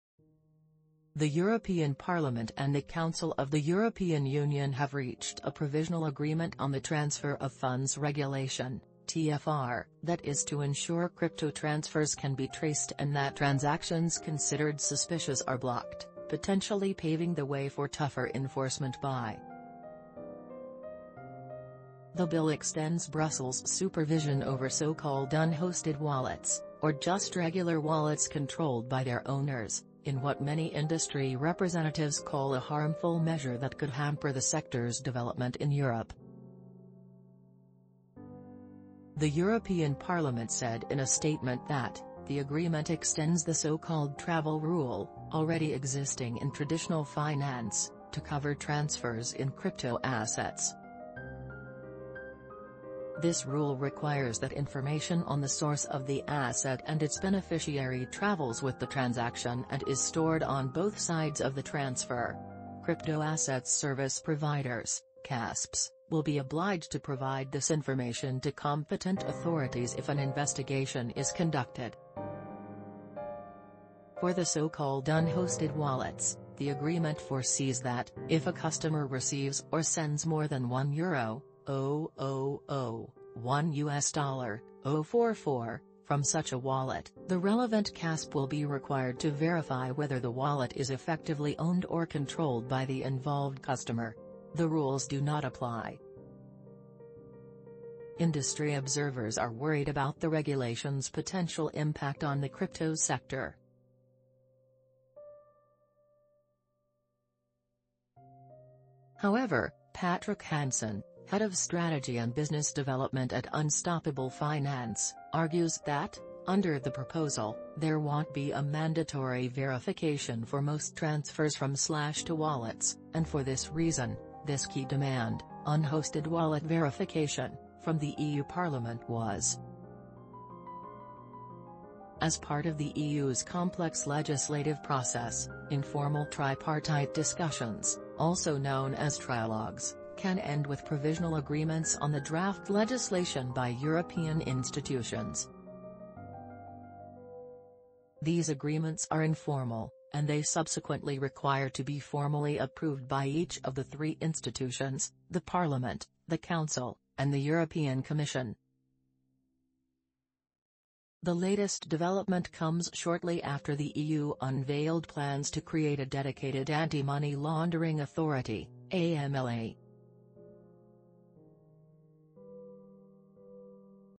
EU Institutions Reach Provision Agreement On Controversial 'Unhosted
The European Parliament and the Council of the European Union have reached a provisional agreement on the Transfer of Funds Regulation (TFR) that is to ensure crypto transfers can be traced and that transactions considered suspicious are blocked, potentially paving the way for tougher enforcement by the European Union. The bill extends Brussels’ supervision over so-called 'unhosted wallets', or just regular wallets controlled by their owners, in what many industry representatives call a harmful measure that could hamper the sector’s development in Europe. The  European Parliament said in a statement that,“The agreement extends the so-called 'travel rule', already existing in traditional finance, to cover transfers in crypto assets. This rule requires that information on the source of the asset and its beneficiary travels with the transaction and is stored on both sides of the transfer.”“Crypto-assets service providers (CASPs) will be obliged to provide this information to competent authorities if an investigation is conducted into money laundering and terrorist financing.”Negotiators from both institutions agreed that the set-up of a public register for non-compliant and non-supervised CASPs, with which the EU-based CASPs would not be allowed to engage in trade, will be covered in the bloc’s Markets in Crypto-assets (MiCA) regulation which is currently being negotiated. For the so-called 'unhosted wallets', the agreement foresees that, if a customer receives or sends more than EUR 1,000 (USD 1,044) from such a wallet, the relevant CASP will be required to verify whether the wallet is effectively owned or controlled by the involved customer.“The rules do not apply to person-to-person transfers conducted without a provider, such as bitcoins trading platforms, or among providers acting on their own behalf,” according to the statement. Industry observers are worried about the regulation’s potential impact on the crypto sector. However, Patrick Hansen, Head of Strategy and Business Development at Unstoppable Finance, argues that, under the proposal, there “won't be a mandatory verification” for “most transfers from/to wallets,” and for this reason, “this key demand ('unhosted wallet verification') from the EU Parliament was quite weakened”. As part of the EU’s complex legislative process, informal tripartite discussions, also known as trilogues, can end with provisional agreements on the draft legislation by European institutions. These agreements are informal, and they subsequently require to be formally approved by each of the three institutions: the Parliament, the Council, and the European Commission. The latest development comes shortly after the EU unveiled plans to create a dedicated Anti-money laundering Authority (AMLA). Under the Council’s proposal, the authority is to be enabled to directly supervise certain types of credit and financial institutions. These will include cryptoasset service providers “if they are considered risky.